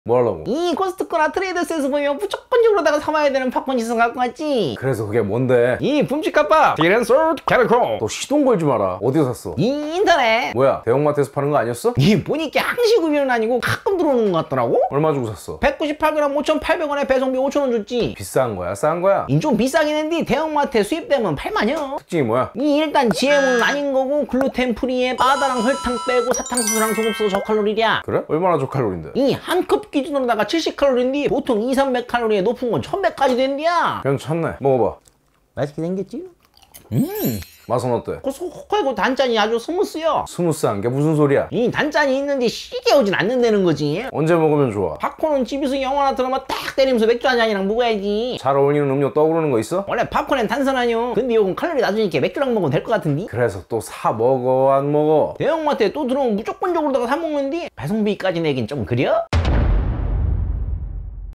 코스트코 트레이더스에서 보이면 꼭 사야하는 팝콘
붐치카팝 스윗앤솔티 리뷰